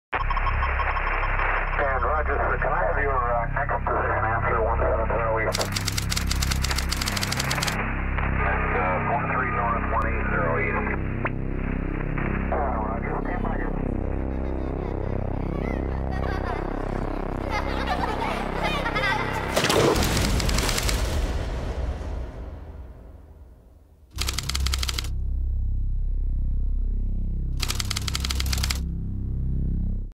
Someone Will Find Them
An award winning PSA prepared by the Canadian Landmine Foundation.